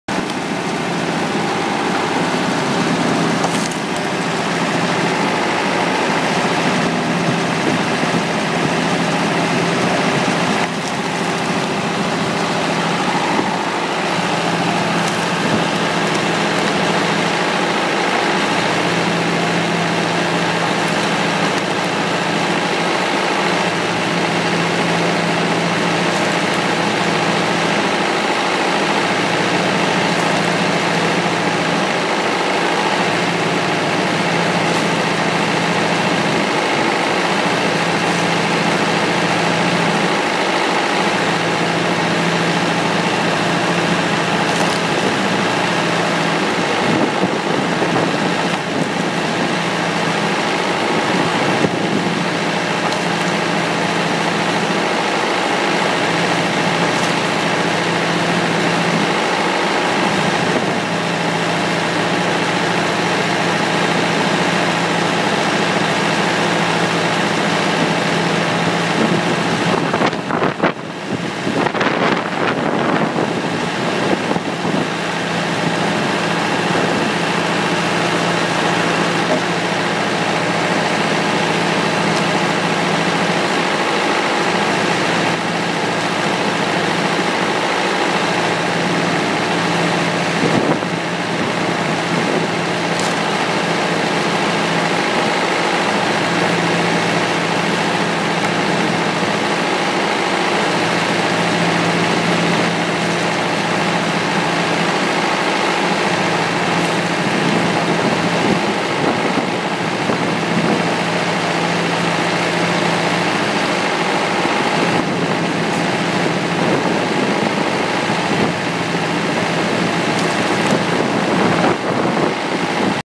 Hogs 4 logs Splitter
Log splitter, 13ton foot operated hands free :-)